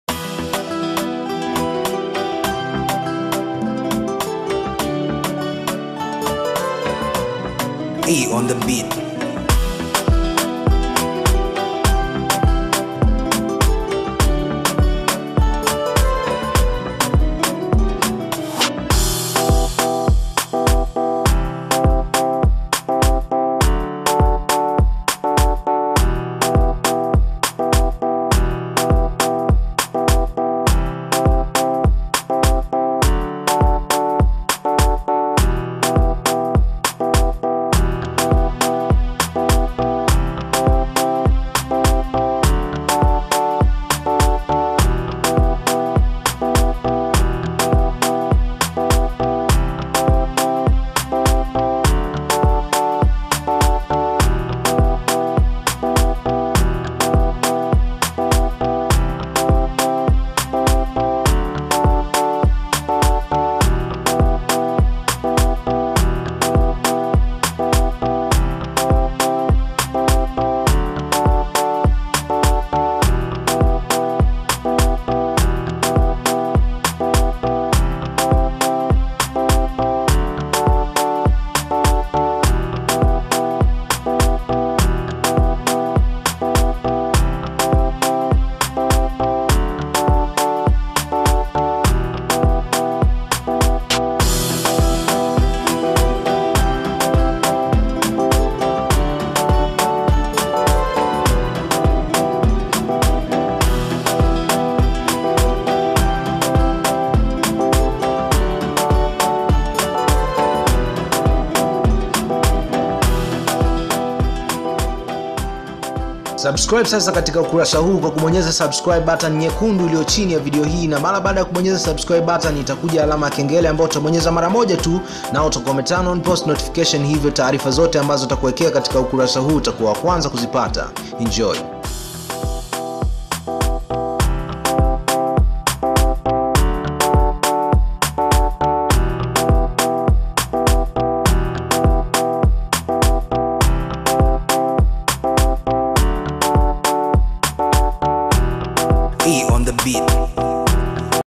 MWANASHERIA AFUNGUKA KUHUSU VIDEO YA NGONO YA MWANADADA MENINA "KESI IKO POLISI"
EDDY ONE TV

MILLARD REPORTER